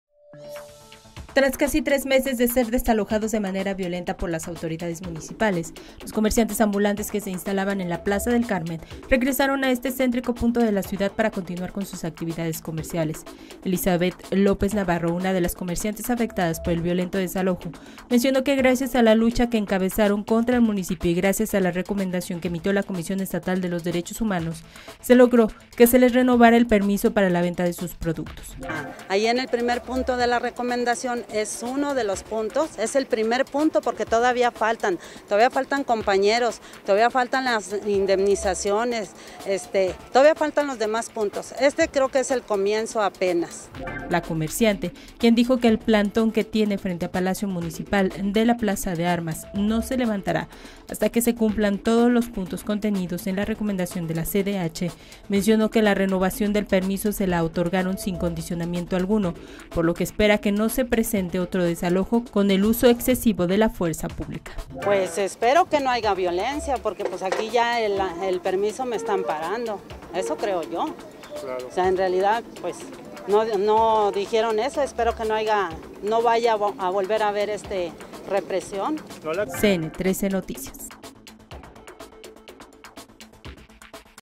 Regresan comerciantes desalojados a Plaza del Carmen
Ayuntamiento no aclara quién realizará operativos, por lo que se presta a irregularidades.